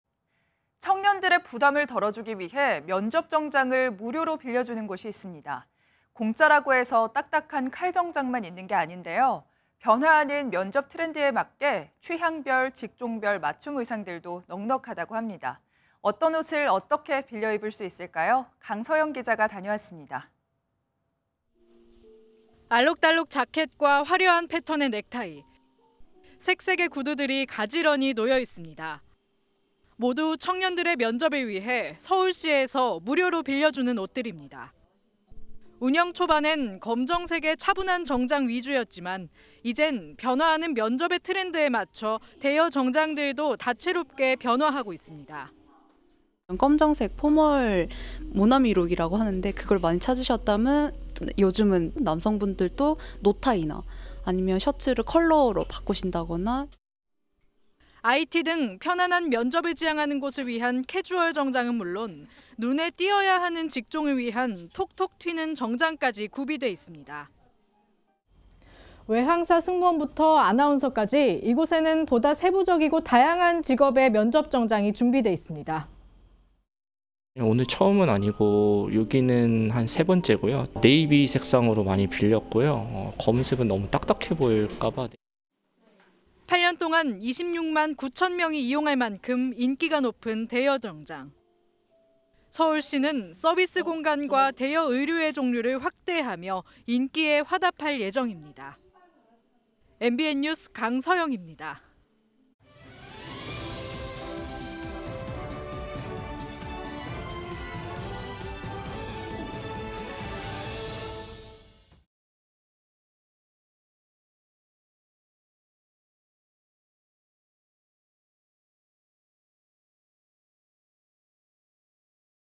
외항사 승무원·아나운서도 OK…변화하는 '대여 정장' [전국네트워크]
【 앵커멘트 】
  청년들의 부담을 덜어주려고 면접 정장을 무료로 빌려주는 곳이 있습니다.
  공짜라고 해서 딱딱한 칼 정장만 갖춰진 것이 아니라 변화하는 면접 트렌드에 맞는 취향별, 직종별 맞춤 의상들도 넉넉하다고 합니다.
  어떤 옷을 어떻게 빌려 입을 수 있을까요?
  강서영 기자가 다녀왔습니다.

【 기자 】
  알록달록 재킷과, 화려한 패턴의 넥타이, 색색의 구두들이 가지런히 놓여 있습니다.

  모두 청년들의 면접을 위해 서울시에서 무료로 빌려주는 옷들입니다.

  운영 초반엔 검은색의 차분한 정장 위주였지만, 이젠 변화하는 면접의 트렌드에 맞춰 대여 정장들도 다채롭게 변화하고 있습니다.

▶ 인터뷰 : 안도빈 / 정장대여 대행업체
- "검정색 포멀 모나미룩이라고 하는데 그걸 많이 찾으셨다면 요즘은 남성분들도 노타이나 아니면 셔츠를 컬러로 바꾸신다거나…."

  IT 등 편안한 면접을 지향하는 곳을 위한 캐주얼 정장은 물론, 눈에 띄어야 하는 직종을 위한 톡톡 튀는 정장까지 구비돼 있습니다.

▶ 스탠딩 : 강서영 / 기자
- "외항사 승무원부터 아나운서까지, 이곳에는 세부적이고 다양한 직업의 면접 정장이 준비돼 있습니다."

▶ 인터뷰 : 장병헌 / 취업준비생
- "오늘 처음은 아니고 여기는 한 세 번째고요. 다 네이비 색상으로 빌렸고요. 검은색은 너무 딱딱해 보여서."

  8년 동안 26만 9천 명이 이용할 만큼 인기가 높은 '대여 정장'.

  서울시는 서비스 공간과 대여 의류의 종류를 확대하며 인기에 화답할 예정입니다.

  MBN뉴스 강서영입니다.

  영상취재 : 이동학 기자
  영상편집 : 이범성